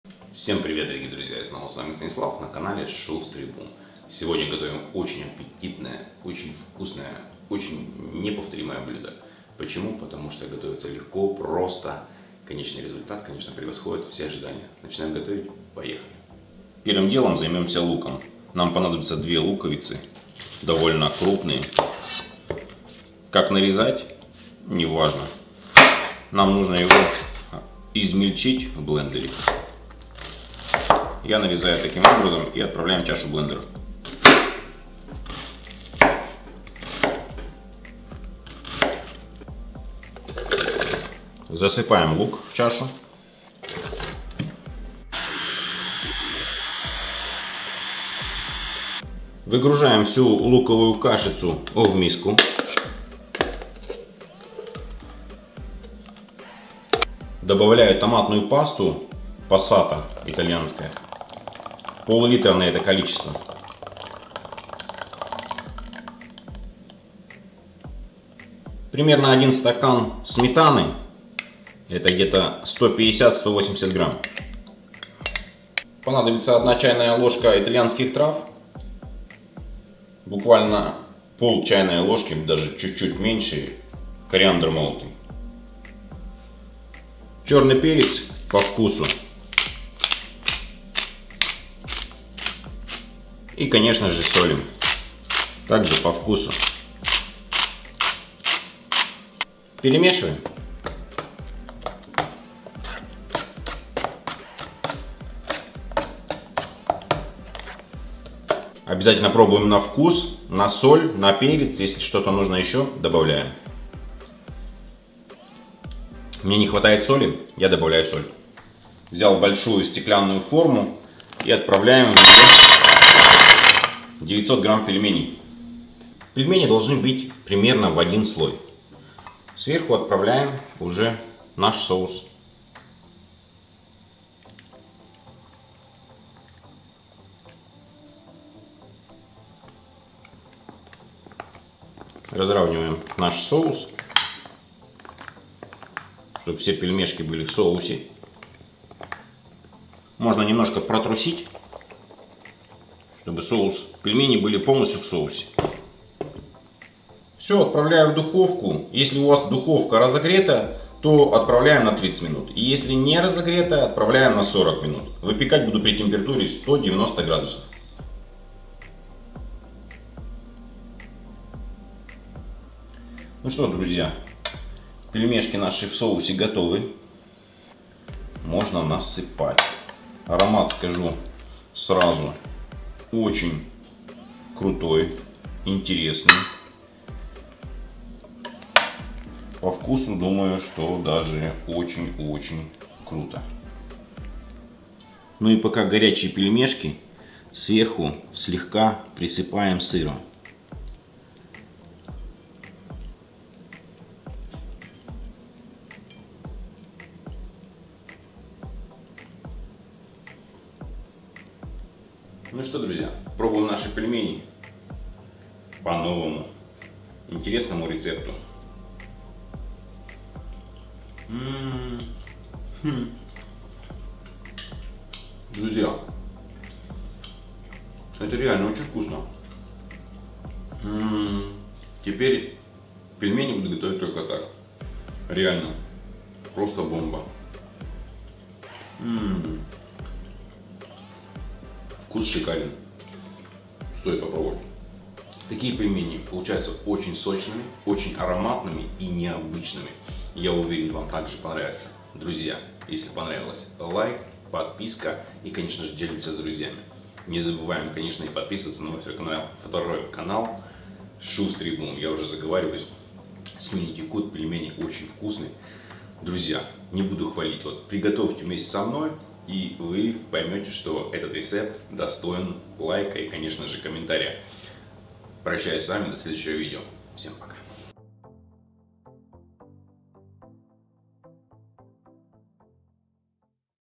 ПЕЛЬМЕНИ станут дефицитом, когда все узнают об этом рецепте! Вот это я понимаю рецепт!!!
ПЕЛЬМЕНИ станут дефицитом, когда все узнают об этом рецепте! Вот это я понимаю рецепт!!! Хороший рецепт найти сложно, но я его нашел!
Невероятно вкусно и просто.

ИНГРЕДИЕНТЫ.
пельмени 900 гр.
томатная паста пассата 500гр
сметана 180гр.
лук 2шт.
итальянские травы
кориандр молотый
черный перец
соль
сыр